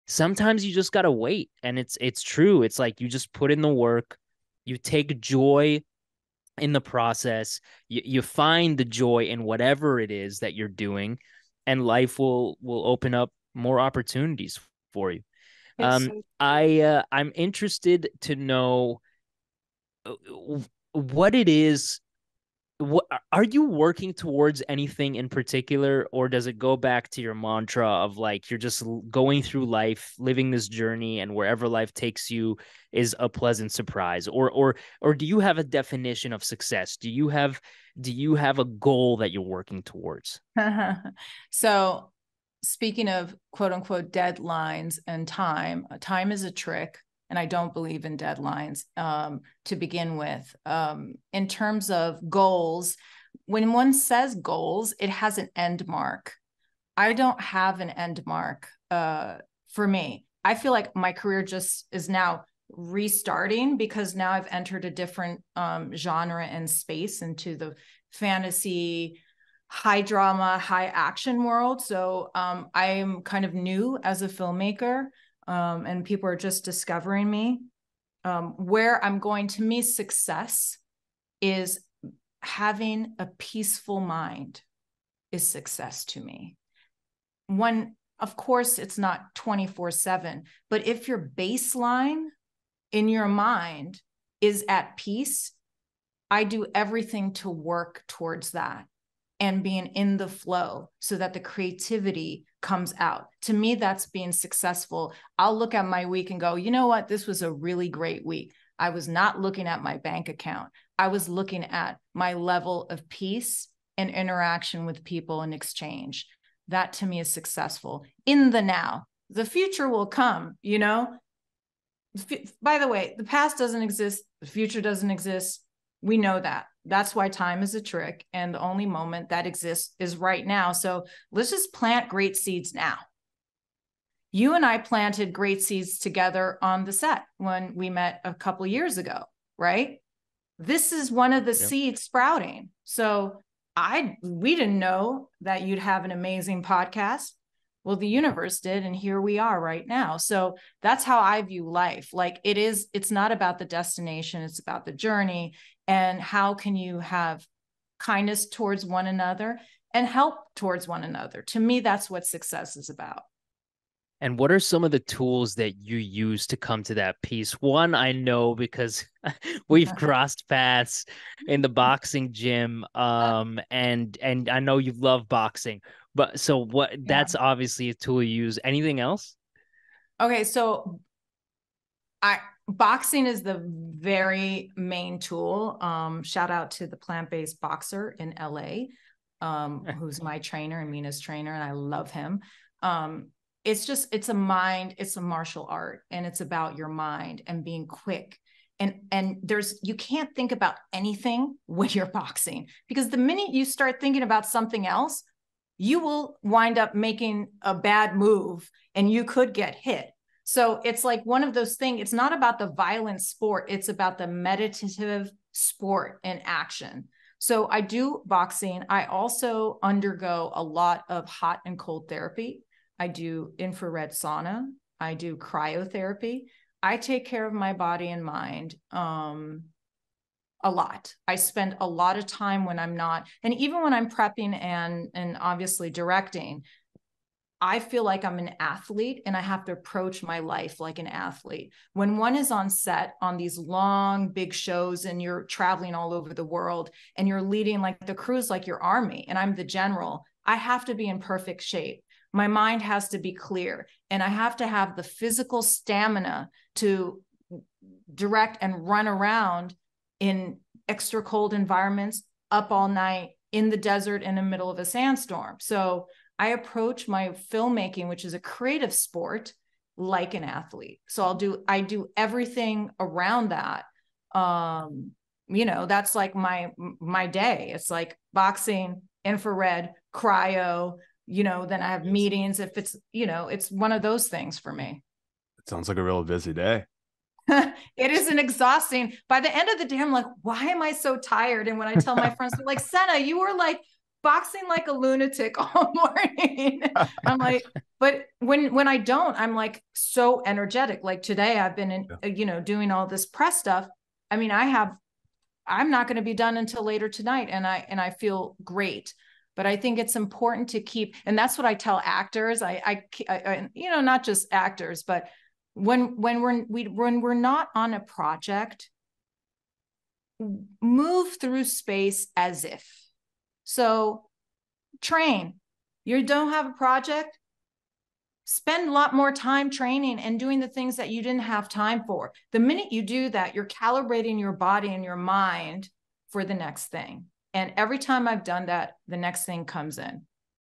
"Success is having a Peaceful Mind" - Sanaa Hamri on How she Manages her Mindset in Hollywood
In the sixth episode of Growth Untold we sat down with Sanaa Hamri who has played a transformative role in the visual and audio world of entertainment. From the melodies of Mariah Carey to the legendary tunes of Prince, she has directed music videos for icons such as Christina Aguilera, Sting, Mary J. Blige, Alicia Keys and more.

Venturing from music onto the screen, she’s also taken us on heartfelt journeys with films such as ‘The Sisterhood Of The Traveling Pants 2’ and led us through love and basketball with 'Just Wright '. And if that wasn't enough, more recently, she’s woven her directorial and executive prowess into the hit TV series 'The Wheel Of Time'. We hope you enjoy!